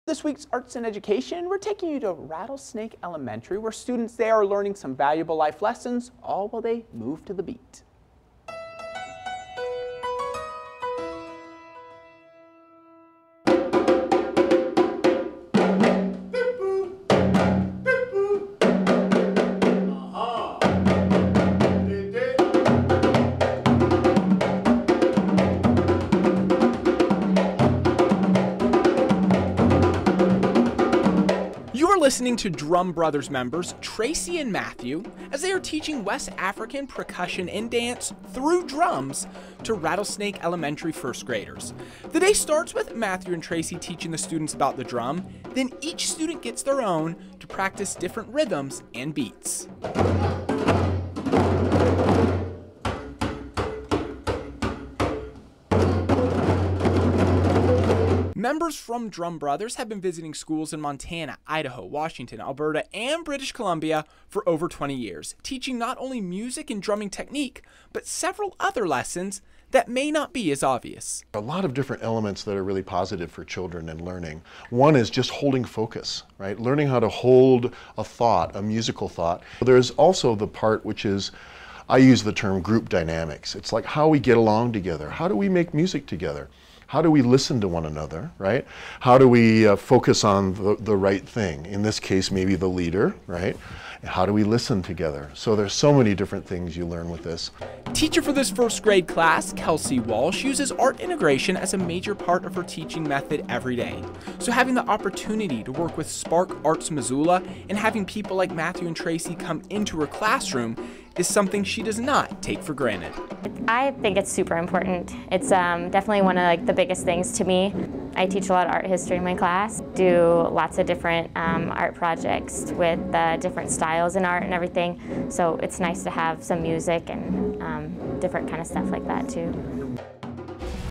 Arts & Education: Missoula students learning to move with the beat
Drum Brothers members Tracey and Matthew have been teaching West African percussion and dance through drums to first grade students at Rattlesnake Elementary School.